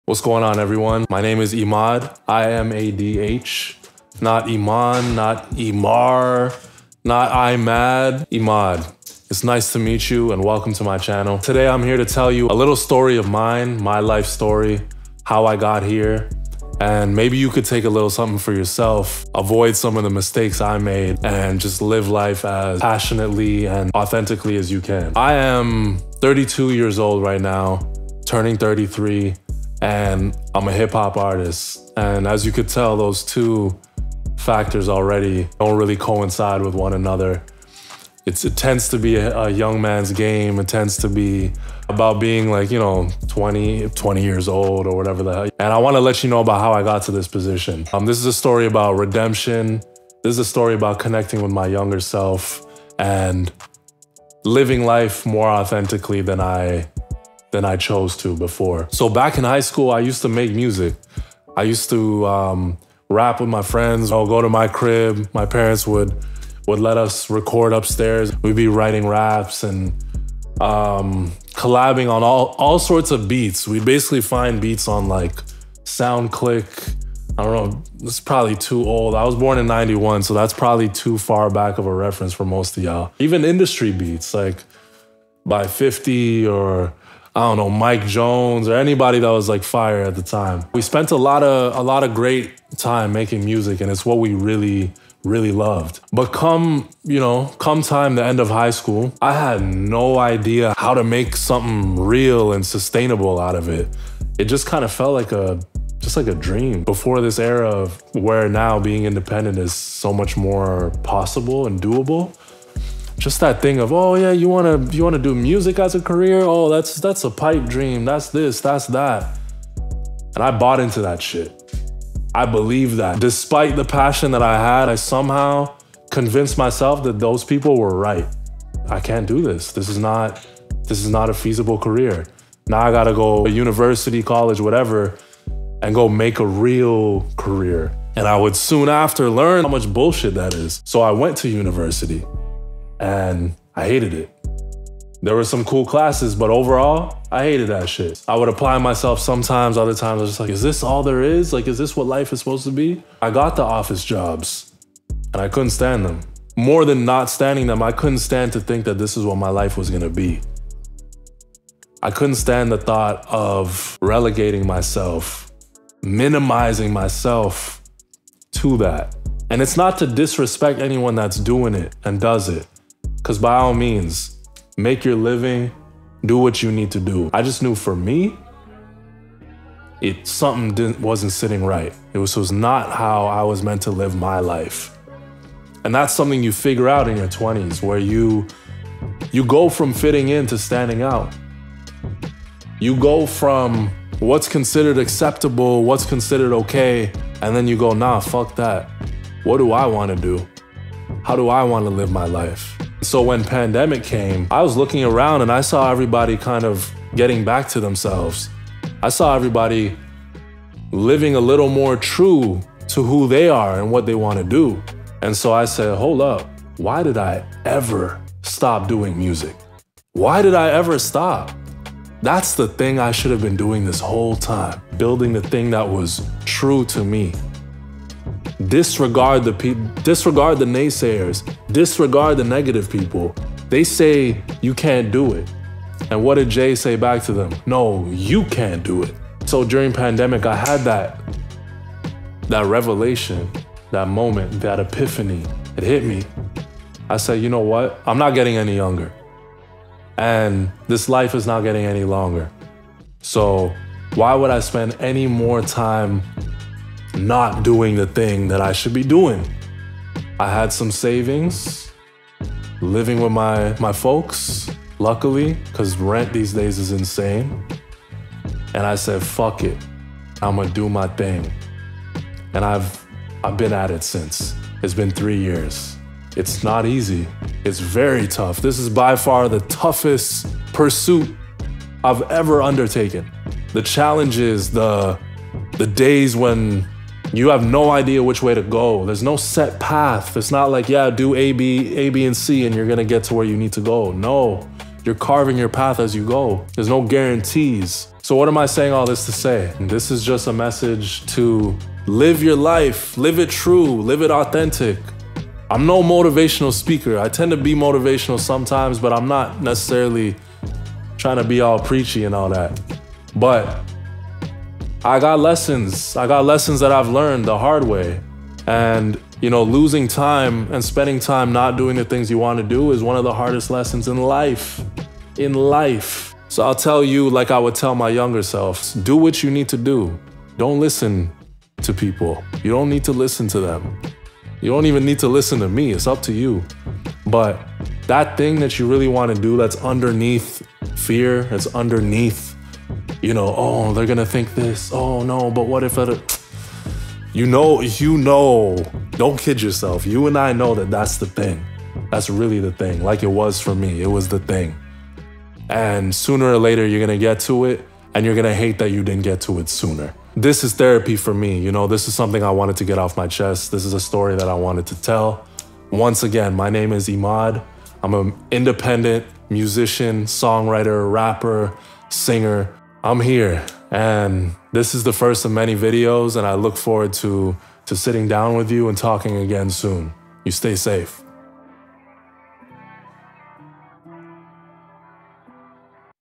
Pursuing a music career in my 30's
This is a story I have been excited and nervous to tell. My name is Imadh and I am an independent music artist. I've been building my professional music career for 3 years now.

Society tells us a lot about how to live our lives and what success looks like. This video is for you if you want to redefine the way you look at success. More importantly, this video is for you if you want to live life more true to yourself and your own standards.

You can find me on all streaming platforms under Imadh and @ImadhMusic on all socials